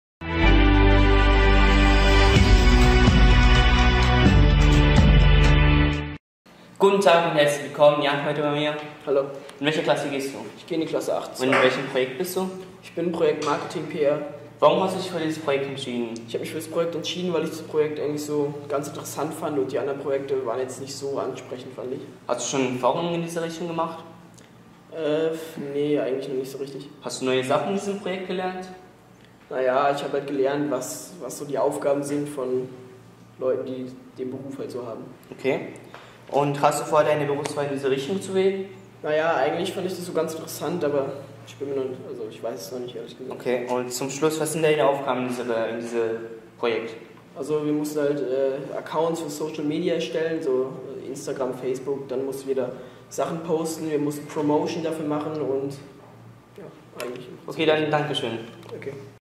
Interview mit Dimitri und Jan
8 Jahrgangsstufe der Grünauer Schule organisiert Fashion Event

Grünau – Im Rahmen der Projektwoche Let’s Move it organisieren die Schüler der 8ten Jahrgangstufe der Grünauer Schule ein Fashion Event unter dem Motto: „Flow Motion – A new Energy Wave“

Am Freitag den 15.07.16 findet um 16Uhr eine Modenschau statt. Zur Vorbereitung dieses Events arbeiten die Schüler der 8ten Klassen die ganze Woche in unterschiedlichen Gruppen wie zum Beispiel, Marketing, Foto, Film, Mode und Event.
Das Ziel ist es am Freitag die Aula der Schule mit Gästen zu füllen und die selbst kreierten Kleidungsstücke erfolgreich vorzuführen.

Pressekontakt:
Marketing Team „FlowMotion“